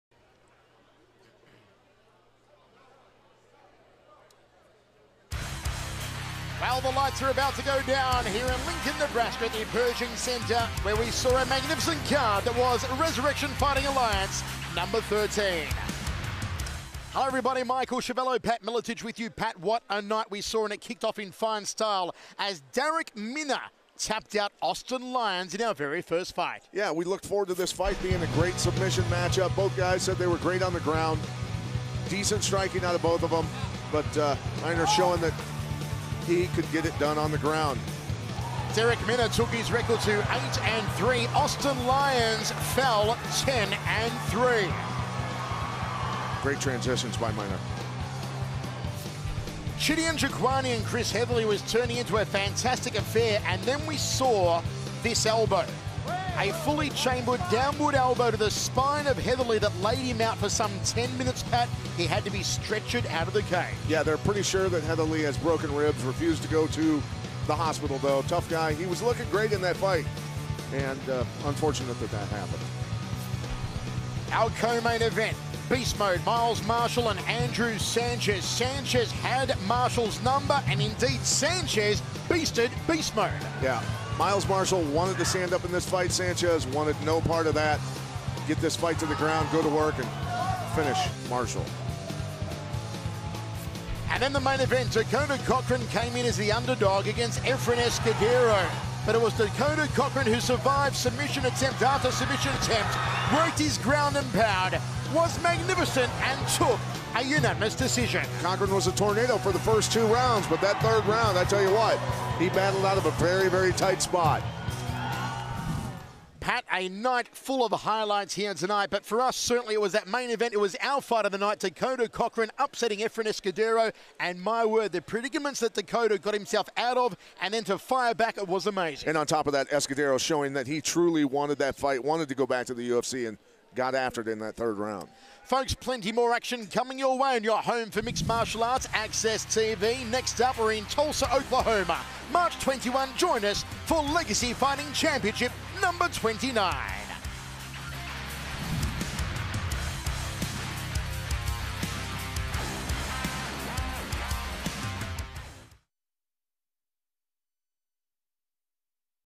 Dakota Cochrane and Efrain Escudero Go to War at RFA 13
RFA 13 is in the books and Dakota Cochrane left the hometown fans smiling. Andrew Sanchez earned himself a title shot and Chidi Njokuani saw his fight with Chris Heatherly end in a controversial No Contest.